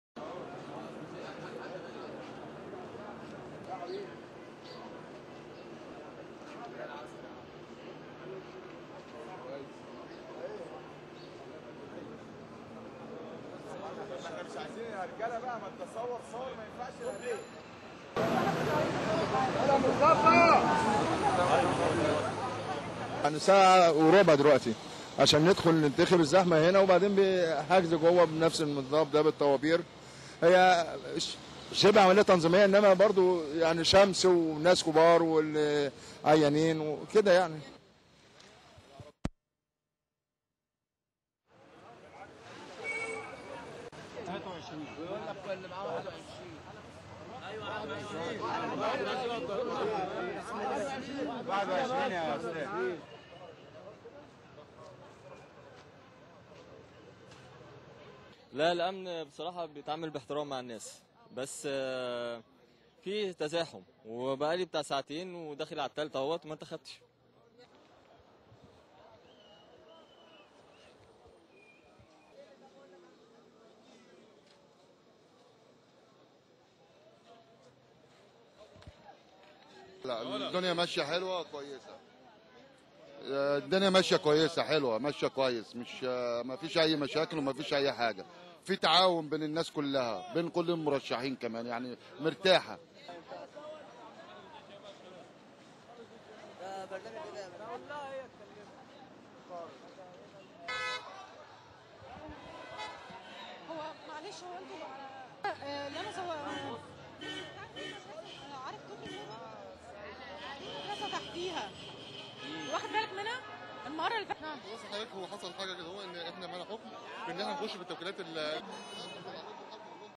اقبال من الناخبين بالجيزة للتصويت
اقبال من الناخبين بالجيزة للتصويت في الانتخابات البرلمانية turnout of voters in Giza in parliamentary elections